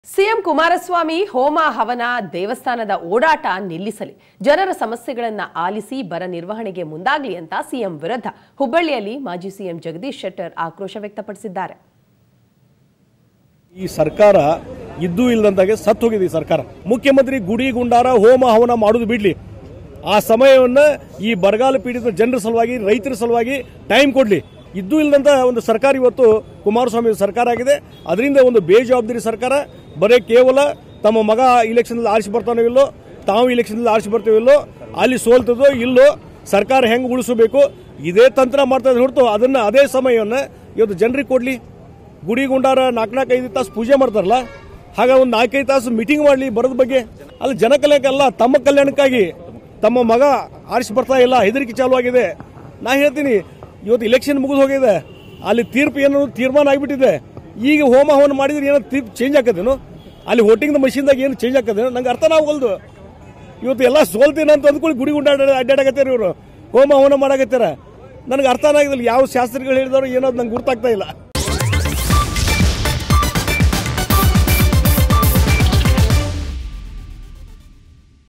ಹೋಮ ಮಾಡಿಸಿದ್ರೆ ಮಷಿನ್‌ನಲ್ಲಿನ ಮತ ಚೇಂಜ್ ಆಗುತ್ತಾ? Jagadish Shettar Outrage Against CM Kumaraswamy
ಹೋಮ ಮಾಡಿಸಿದ್ರೆ ಮಷಿನ್‌ನಲ್ಲಿನ ಮತ ಚೇಂಜ್ ಆಗುತ್ತಾ? Jagadish Shettar Slams CM Kumaraswamy For Visiting Temples